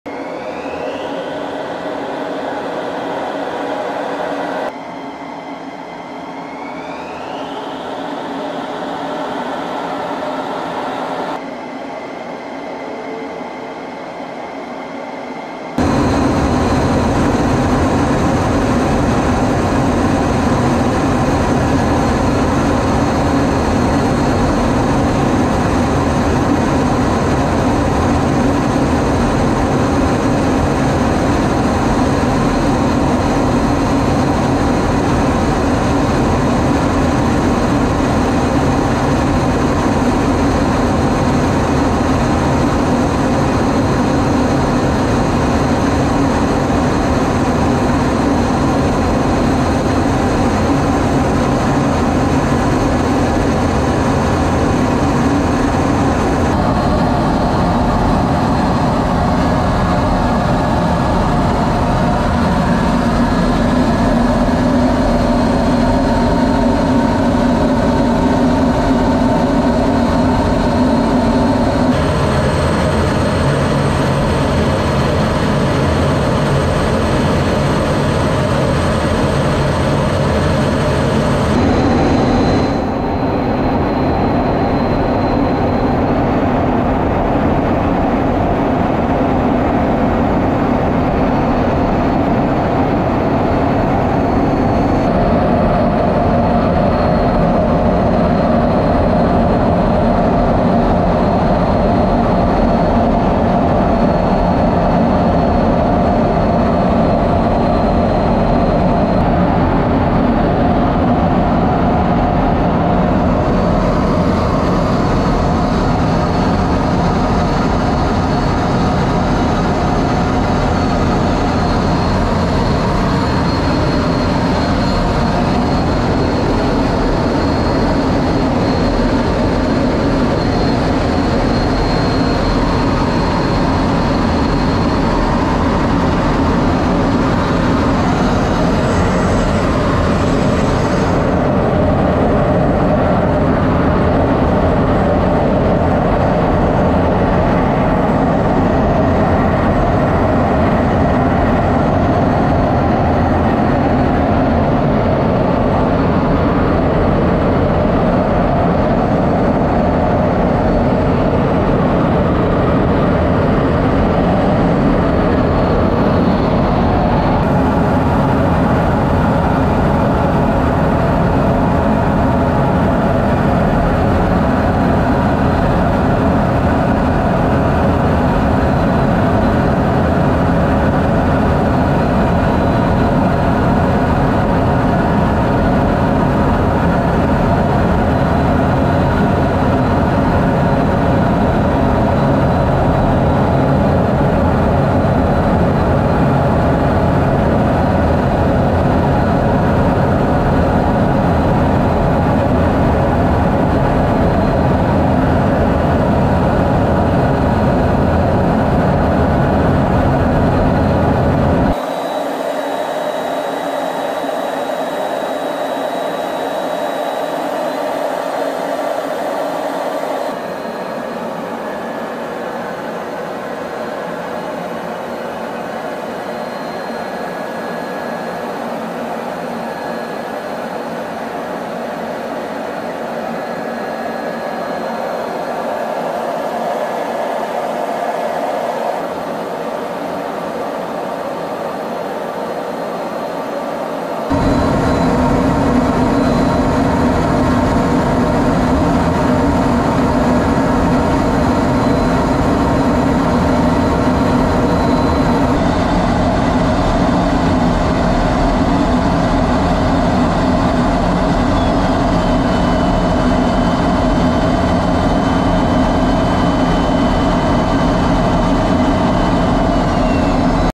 64th Aggressor Point of View at RED FLAG 22-1
Footage from inside the cockpit of an F-16 Fighting Falcon assigned to the 64th Aggressor Squadron during Red Flag 22-1.

64th Aggressor Point of View at RED FLAG 22-1
LAS VEGAS, NV, UNITED STATES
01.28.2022

Film Credits: Video by Tech. Sgt. Alexandre Montes